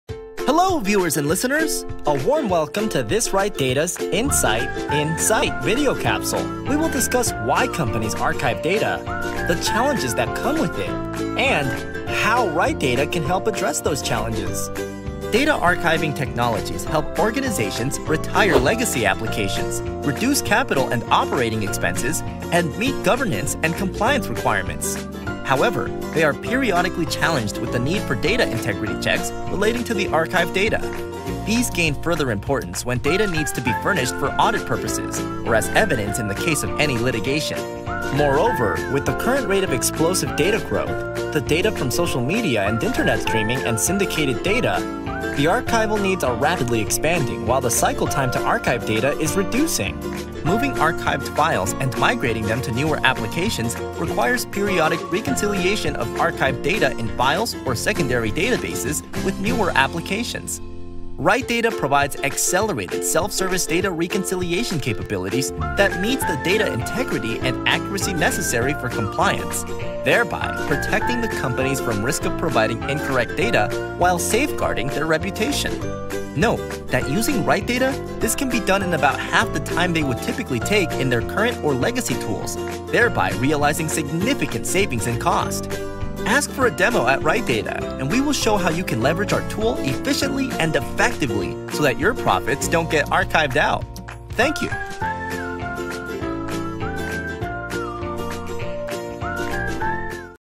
RightData For Data Archival Integrity Validation
This is a use-case overview video for Data Integrity Validation for Data Archival process using RightData.